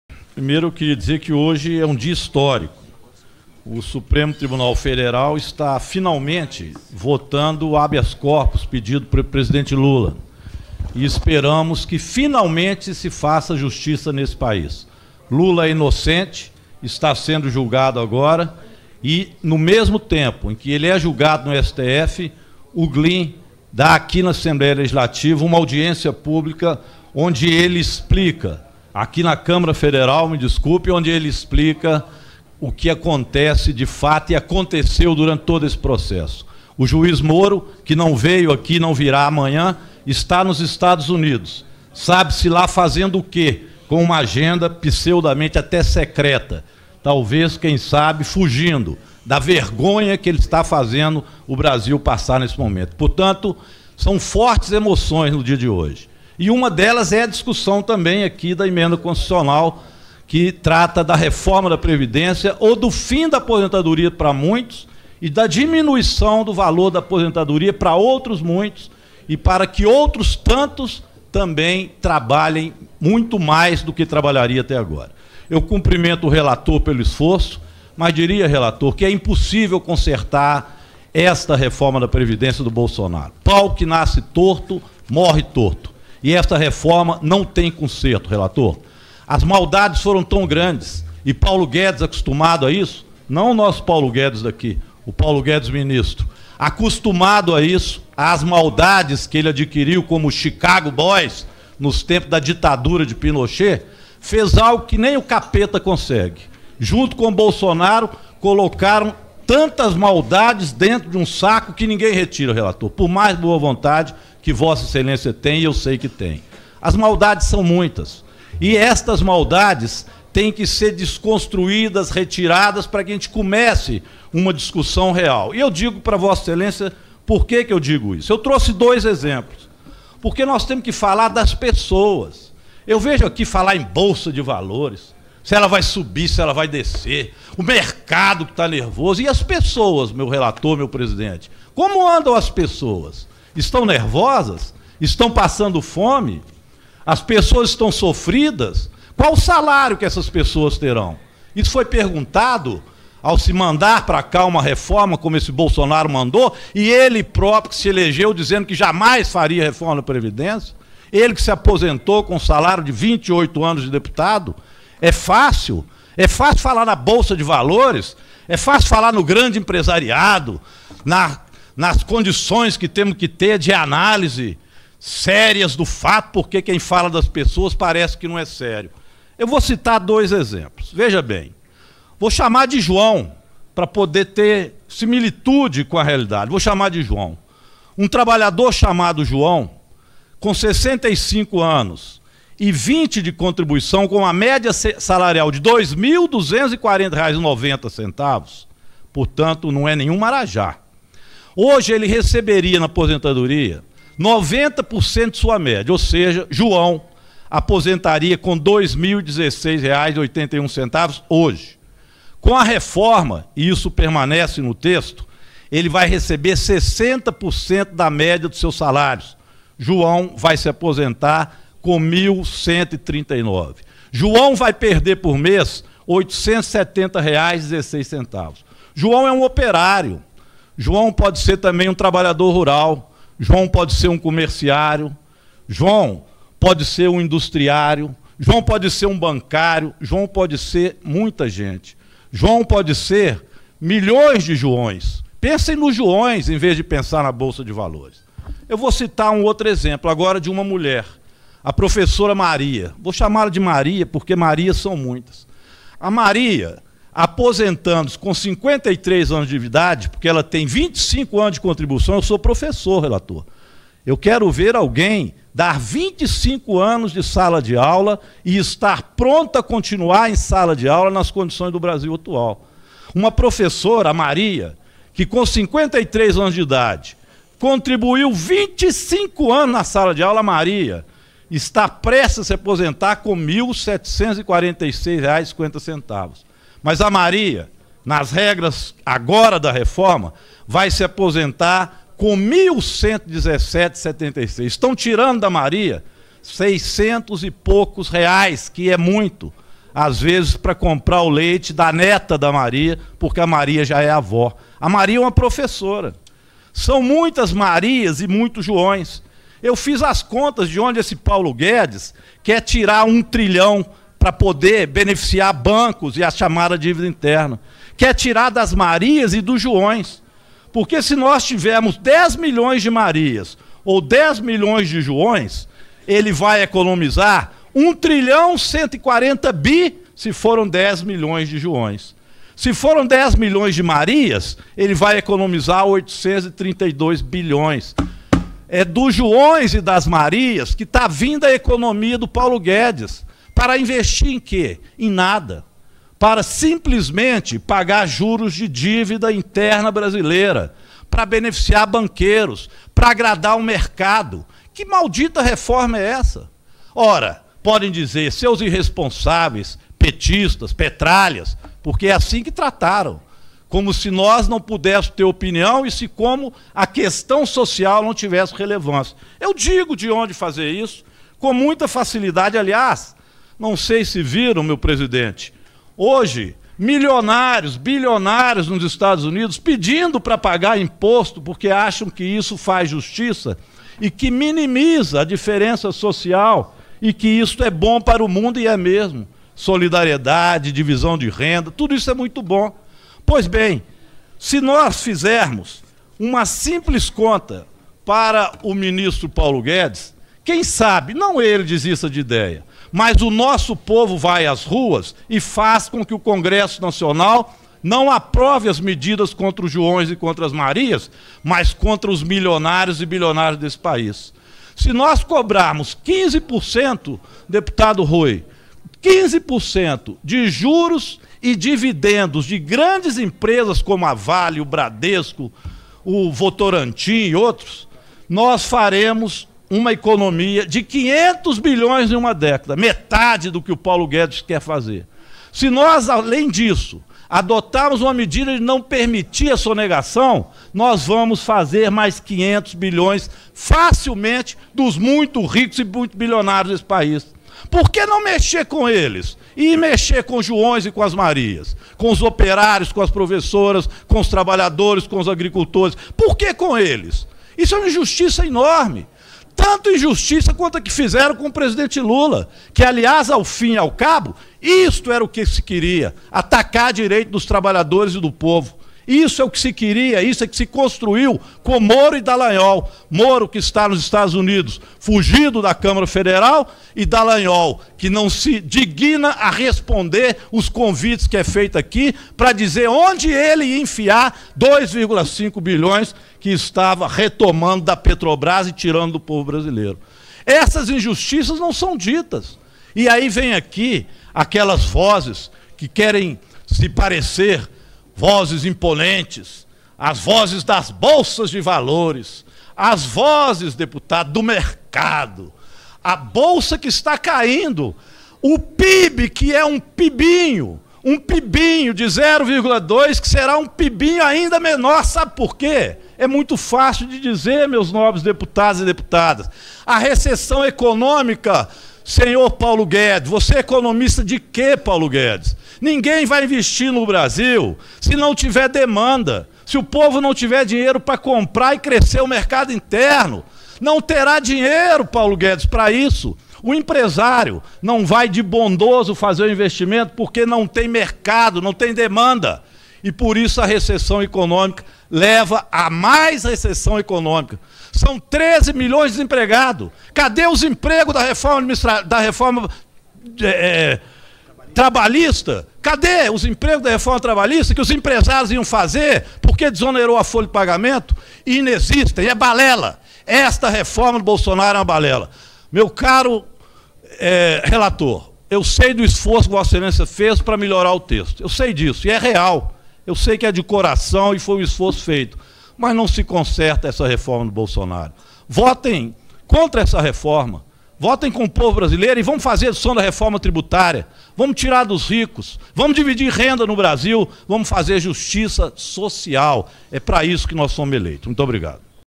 Defensores da reforma da Previdência só falam em mercado e bancos. E as pessoas?
Vale a pena assistir a esta fala do deputado federal Rogério Correia (PT-MG) feita há pouco na Comissão da PEC da Reforma da Previdência. Ele lembra que os defensores da reforma só falam em mercado financeiro, números de bancos e das bolsas de valores. “E as pessoas, elas não contam?”, questiona o deputado mineiro.

Rogério Correia, deputado federal (PT-MG)